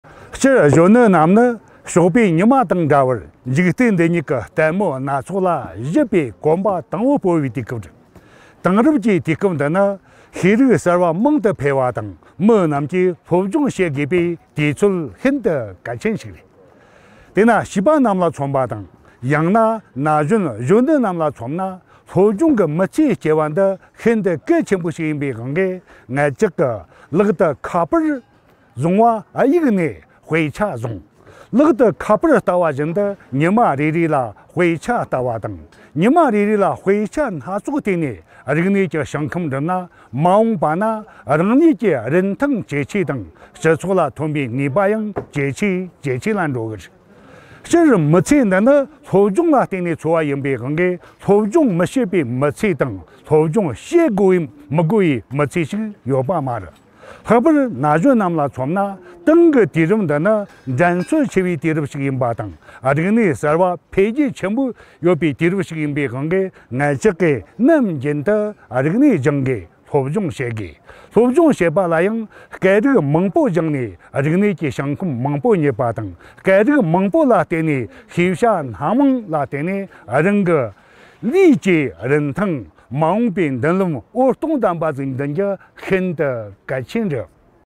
Words sent to Tibetan youth by Professor Kyithek Gya, a great scholar of Tibetan
དགེ་རྒན་ཆེན་མོ་ལྕགས་ཐར་རྒྱལ་ལགས་ཀྱིས་གངས་ཅན་གཞོན་ནུ་རྣམས་ལ་གདམས་པའི་སྙིང་གཏམ།